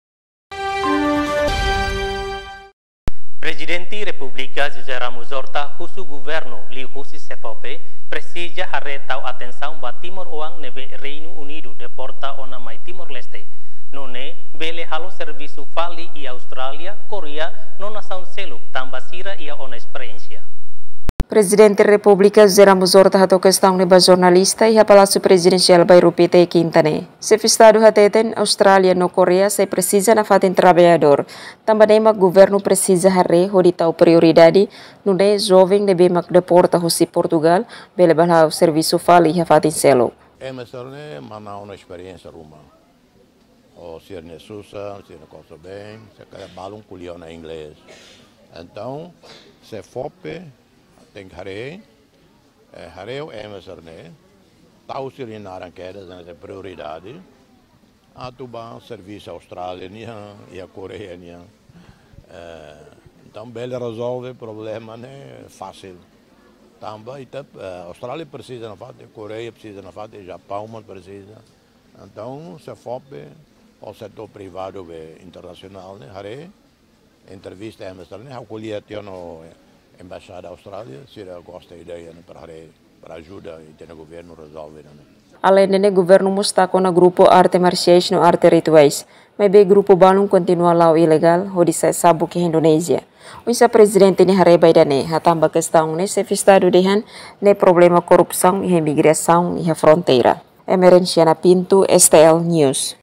PR Horta, Governu Presiza Fo Atensaun Ba Timoroan Neebe Deporta Husi Reinu Unidu
Prezidente Republika, José Ramos Horta, Husu Governu Liu Husi SEFOPE, Presiza Hare Tau Atensaun Ba Timor Oan, Neebe Reinu Unidu, Deporta Ona Mai Timor Leste. Nunee Bele Halo Serbisu Fali Iha Australia, Korea No Nasaun Seluk, Tanba Sira Iha Ona Esperensia.